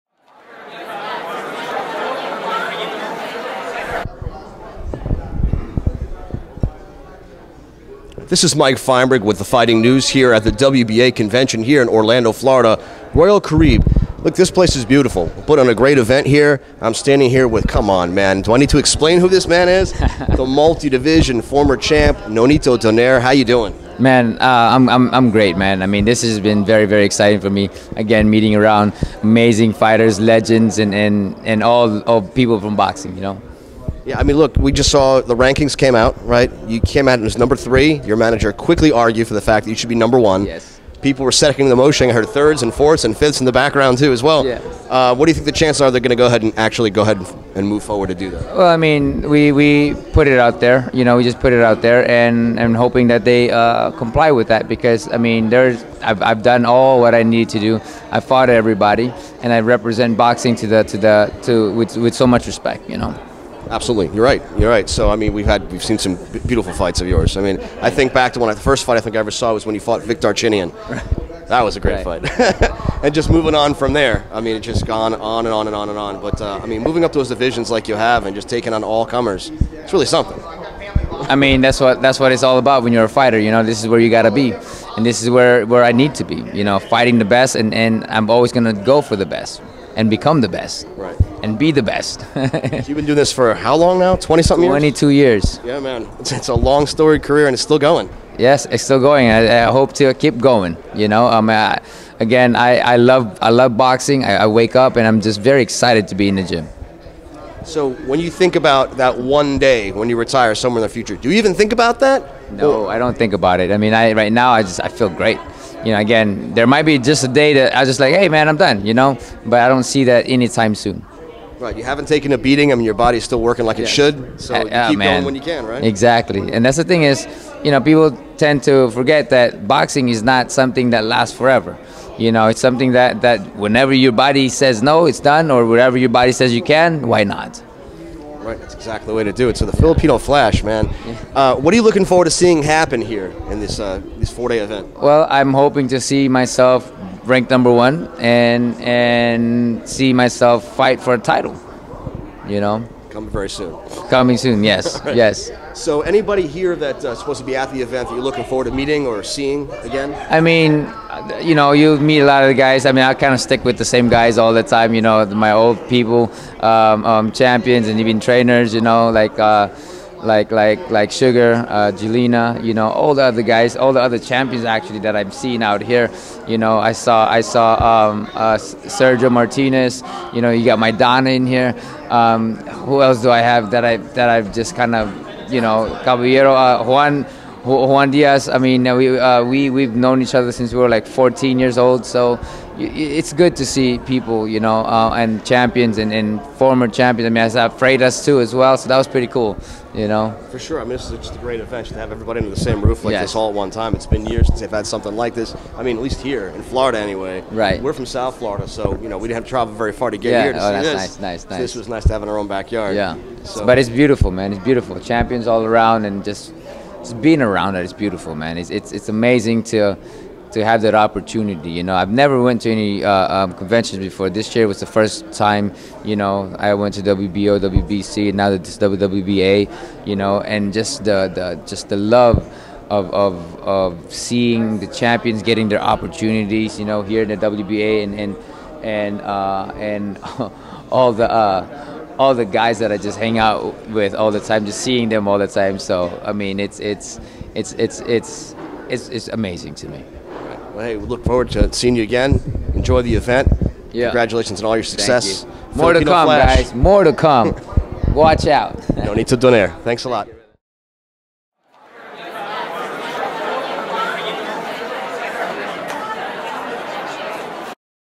WBA rankings just came out and Nonito Donaire only ranked 3rd ? What do you think ?
The Fighting News Mike Feinberg interviewed Nonito Donaire at the WBA 100th year convention at The Caribe Royale Hotel Orlando. @CaribeRoyaleOrlando

Facebook.com/TheFightingNews
Twitter.com/TheFightingNews